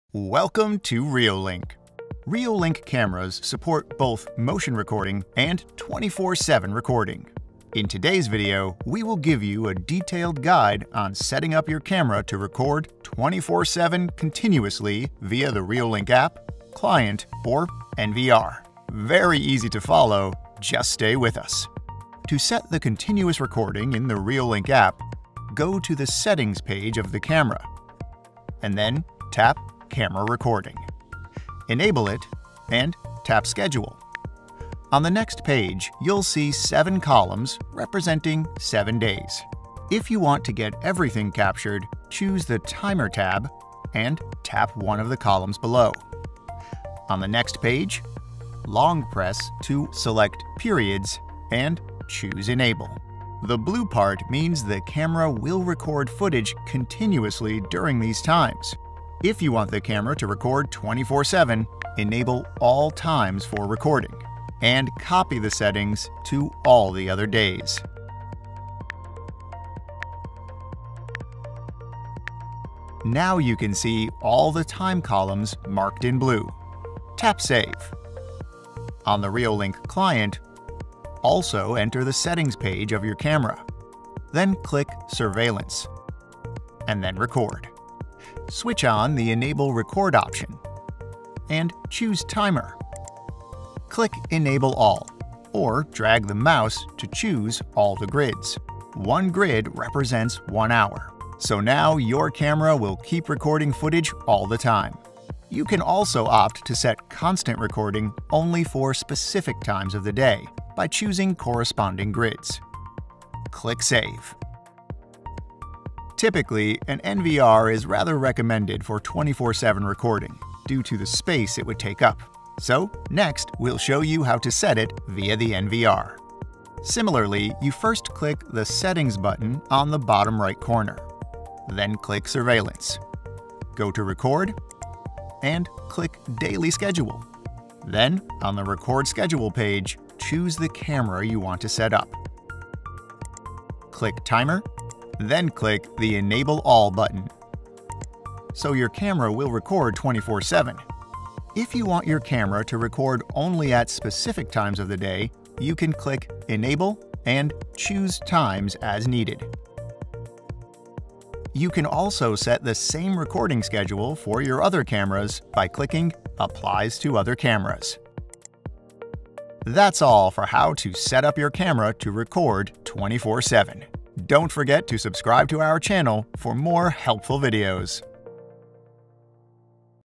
How to Set up 24/7 Continuous Recording (via Reolink App/Client/NVR)
In this video, we’ll give you a detailed guide on how to set up 24/7 continuous recording on your Reolink App, Client or NVR.

24/7 camera recording is a great option for you to get to know everything that happened around your home. It’s also a good way to capture all the memorable moments in your life. This feature is made easy-to-set on your Reolink App or Client, and you can also set it directly via the NVR if you’re using one.

0:00 Intro
0:21 Method 1. Set up 24/7 Continuous Recording via the Reolink App
1:19 Method 2. Set up 24/7 Continuous Recording via the Reolink Client
1:56 Method 3. Set up 24/7 Continuous Recording via the Reolink NVR